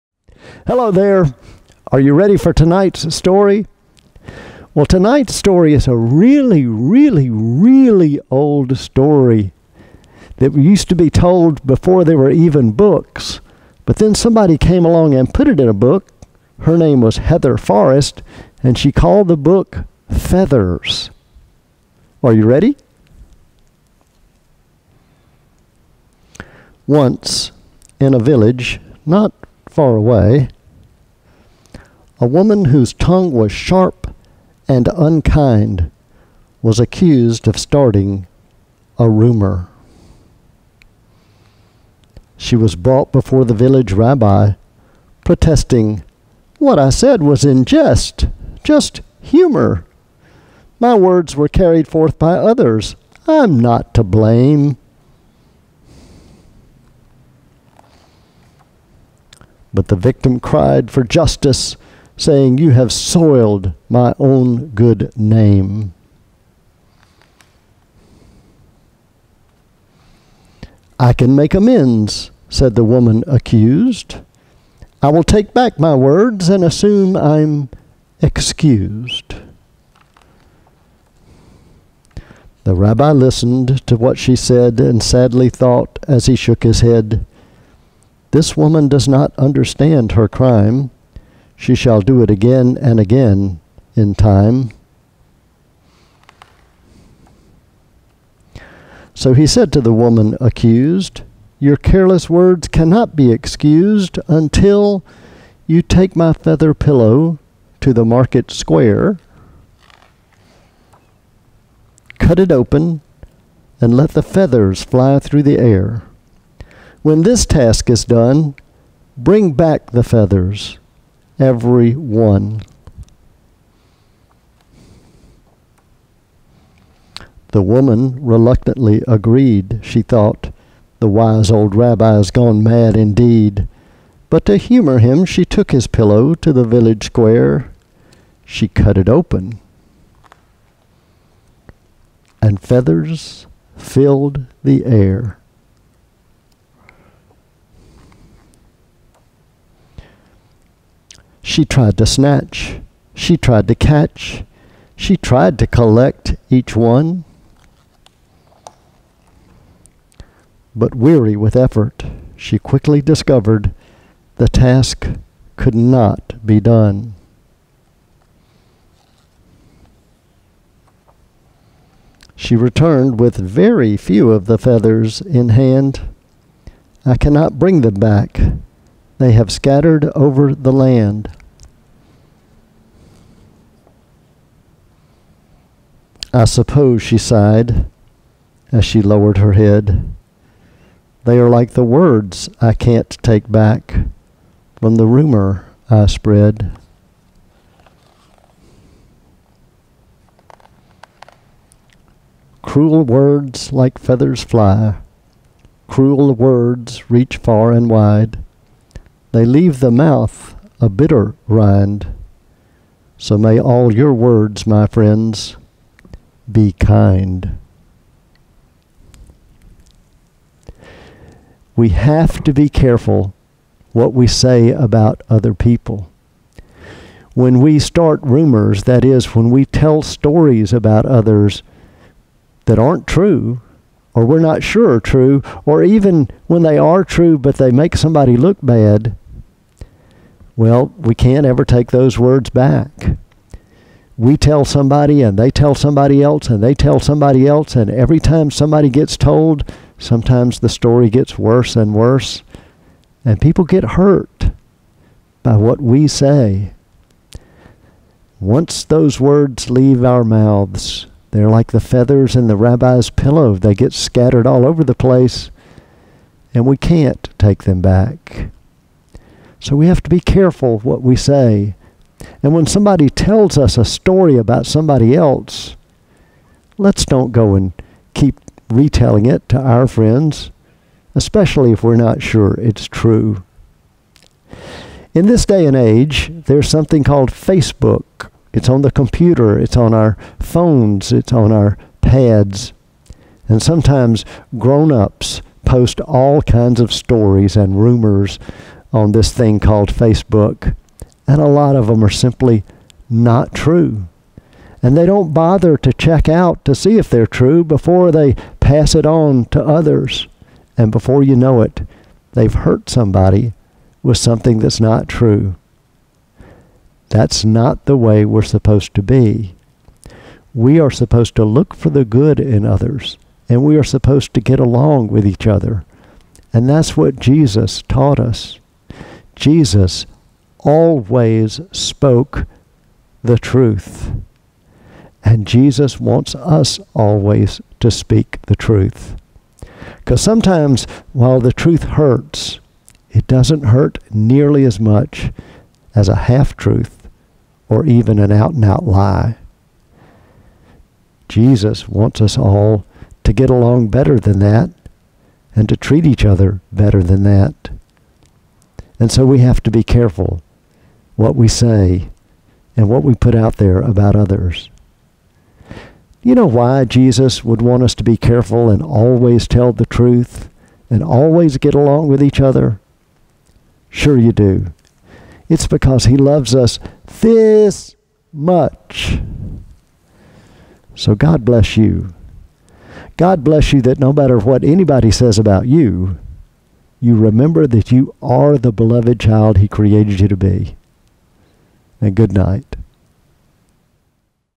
Storytime 95, June 20, 2020: Feathers
A classic European folk tale retold by Heather Forest about trying to get your words back after starting a rumor